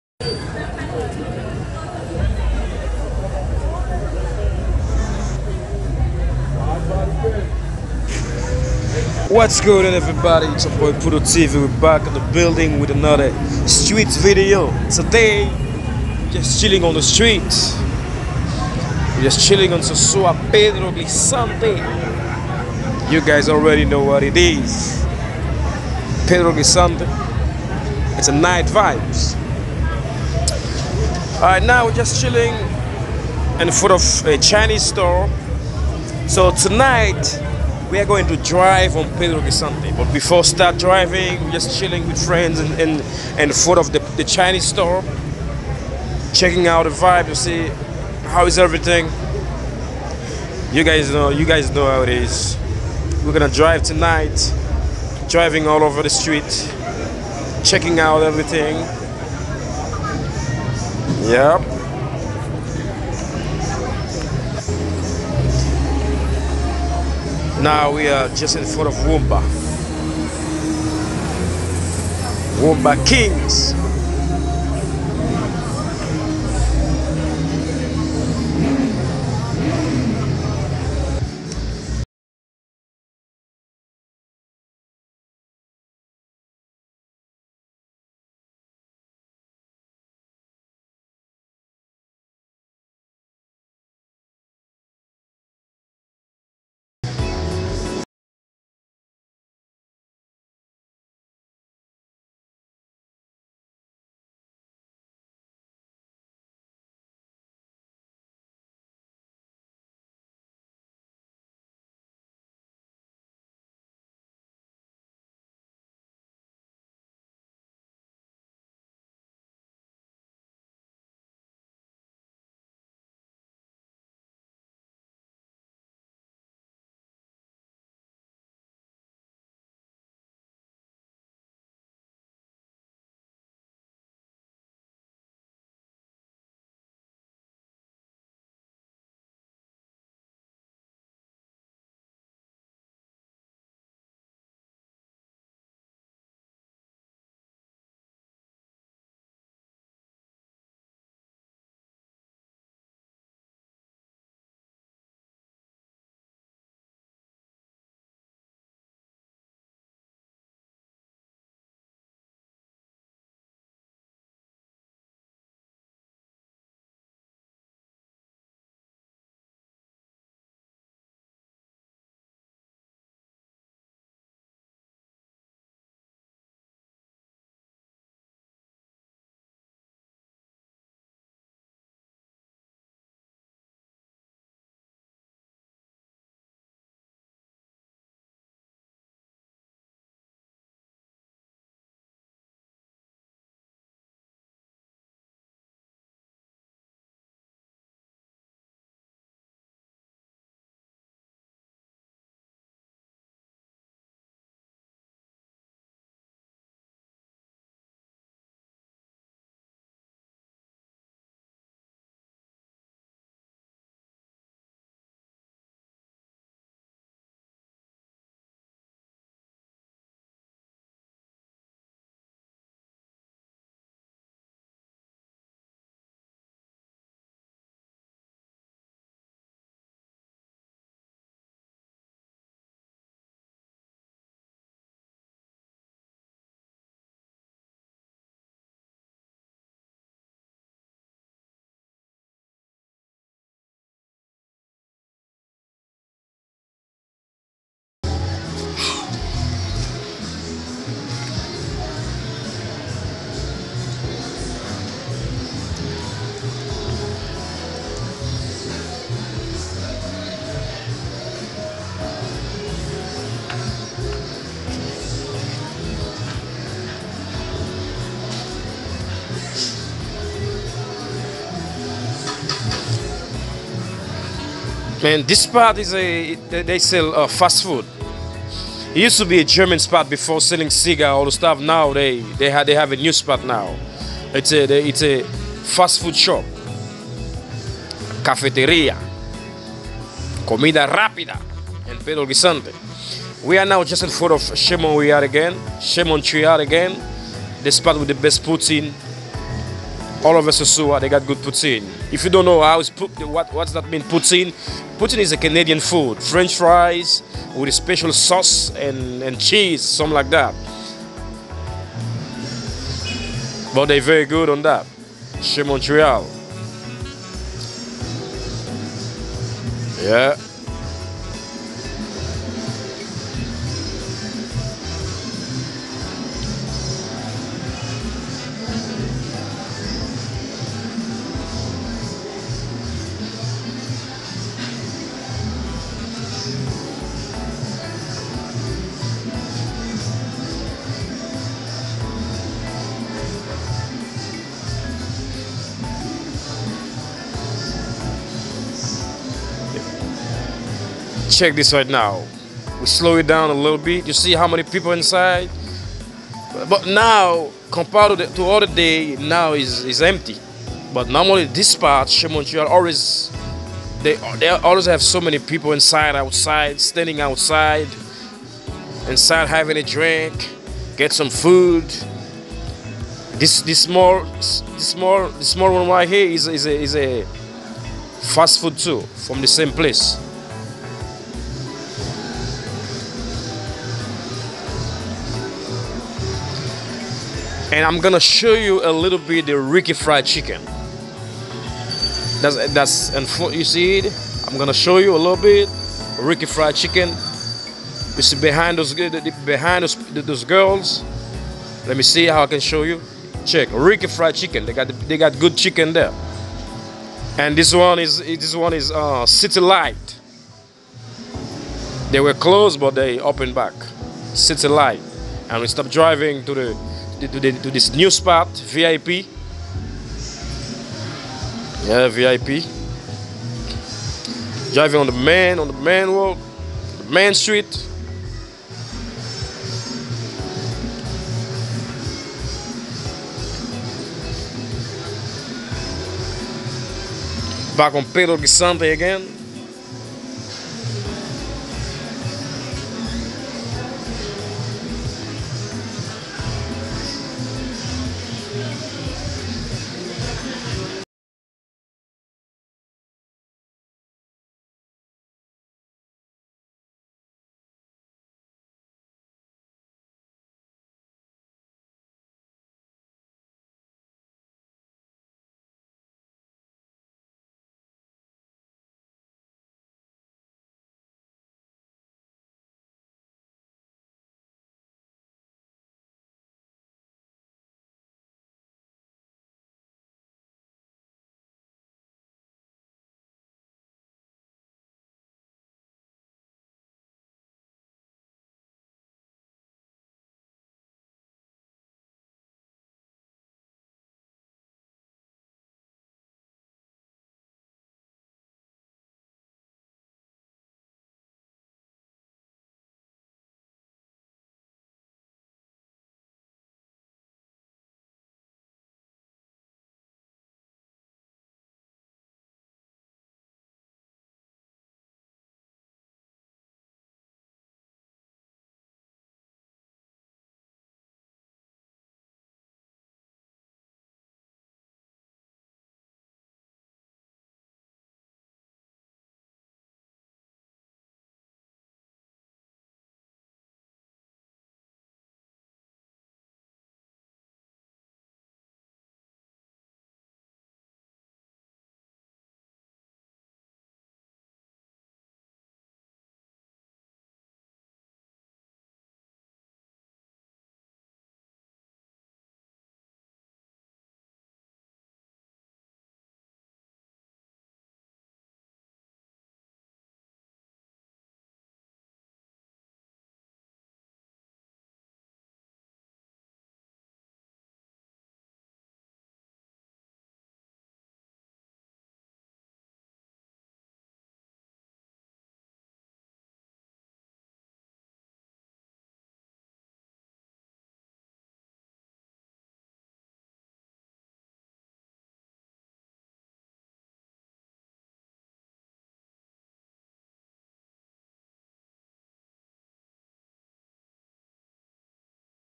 scenic drive,view & businesses in sosua|Ave pedro clisante|dominican republic🇩🇴
Sosúa is a beach town in the Puerto Plata
 province of the Dominican Republic. Located approximately 4 miles from the Gregorio Luperón International Airport in San Felipe de Puerto Plata.